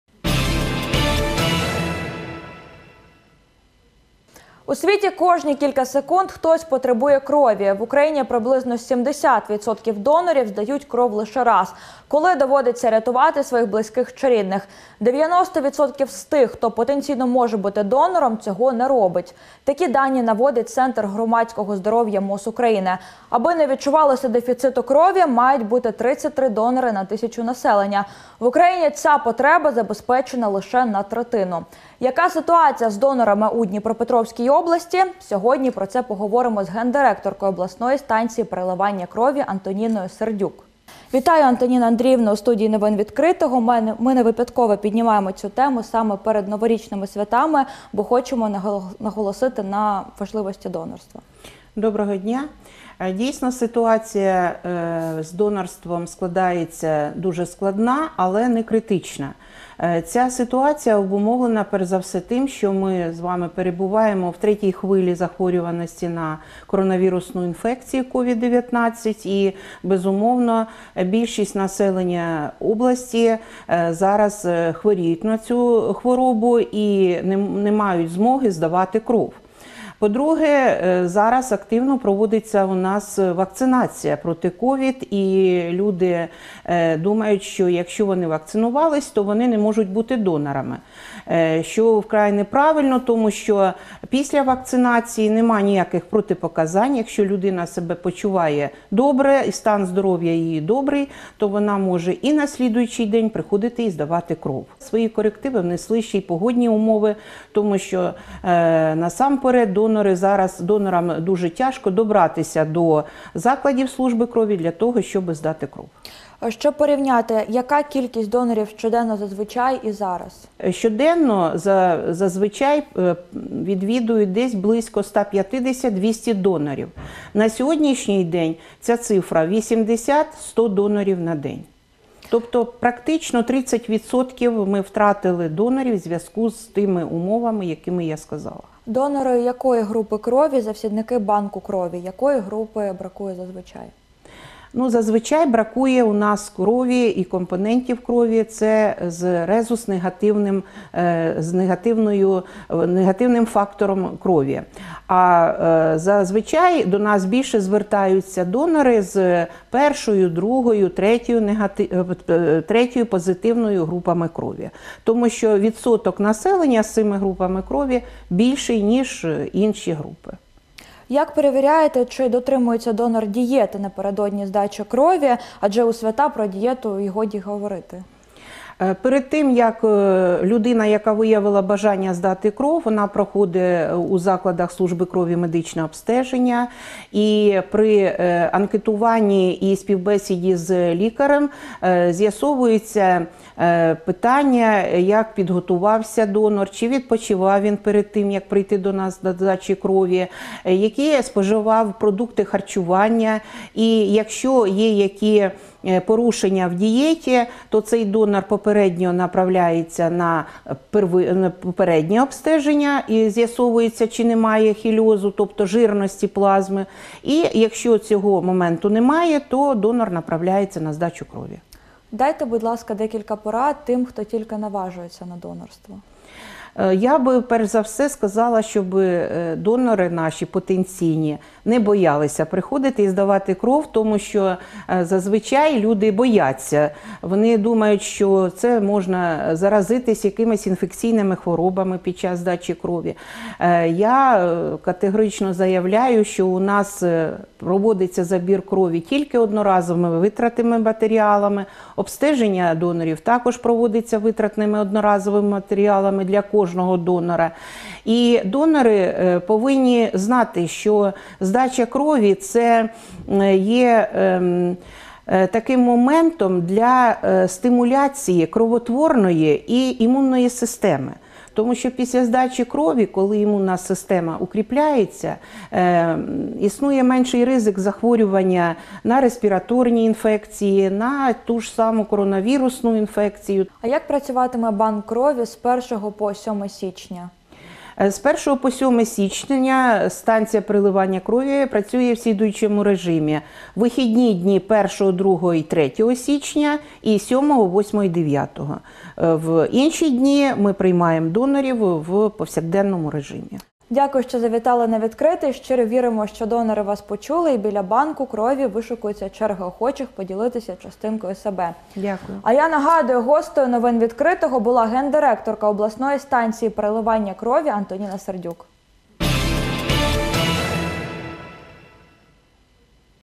Дніпрян просять здати кров перед Новим роком і подарувати шанс на життя
У світі  кожні кілька секунд хтось потребує крові. В Україні приблизно 70 відсотків донорів здають кров лише раз – коли доводиться рятувати своїх близьких чи рідних. 90% з тих, хто потенційно може  бути донором, цього не роблять. Такі дані наводить Центр громадського здоров’я МОЗ України. Аби не відчувалося дефіциту крові, мають бути 33 донори на тисячу населення. В Україні ця потреба забезпечена лише на третину. Яка  ситуація з донорами у Дніпропетровській області? Сьогодні про це поговоримо з гендиректоркою обласної станції переливання крові Антоніною Сердюк.